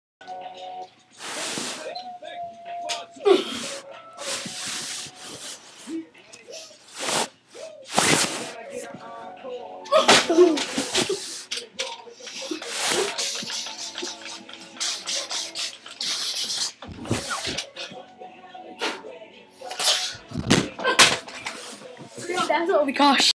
cowboy vs gangster 2 bloopers
it's been a long time since i made these videos and i found this video and it's pretty funny.